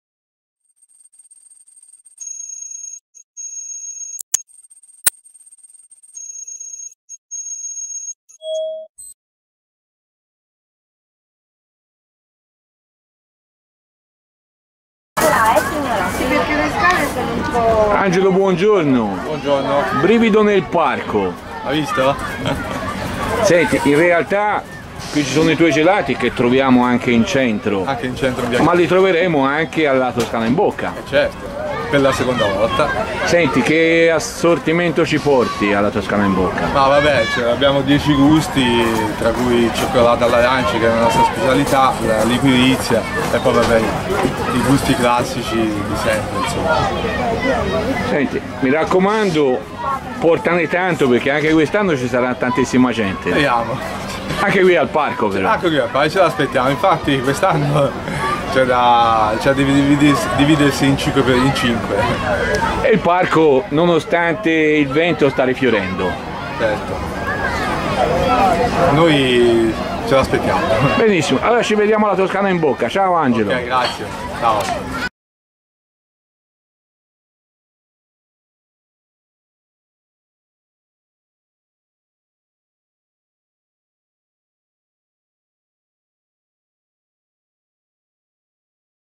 Gelato artigianale a La Toscana in Bocca a Pistoia
Gelato artigianale a La Toscana in Bocca a Pistoia la gelateria Brivido tradizione toscana in tavola nella kermesse enogastronomica gelateria artigianale a Pistoia in via Cavour lato chiesa san Giovanni Fuor Civitas e al Parco della Rana